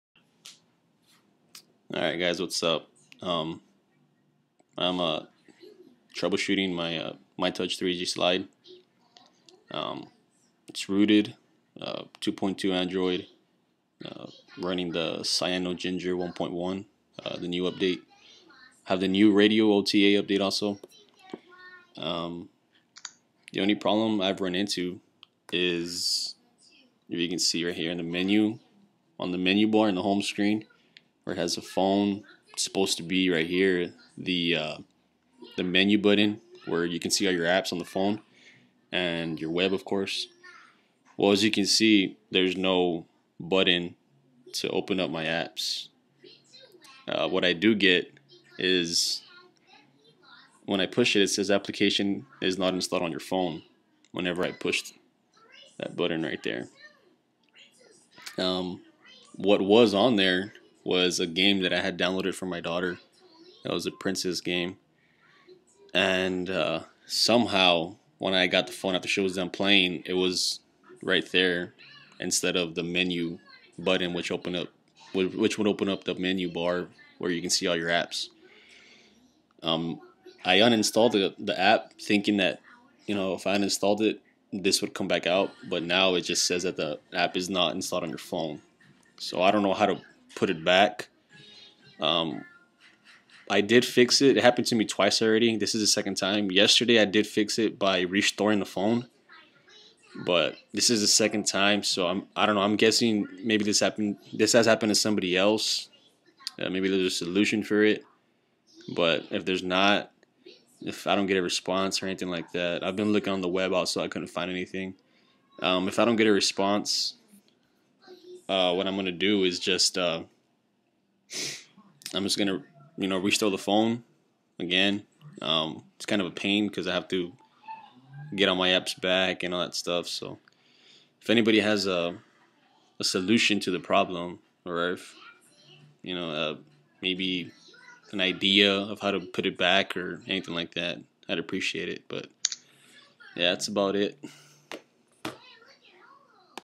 Troubleshooting Mytouch 3G Slide Menu Bar on Home Screen
For some odd reason my phone will not let me open the menu bar and let me see my apps on my phone.  I've tried everything in the settings and I've also looked online and I can't seem to find a solution.  If anybody can help me out I would appreciate it, if not I'm just going to restore my phone using the nandroid which I don't want to do because then I would have to redownload all of my apps.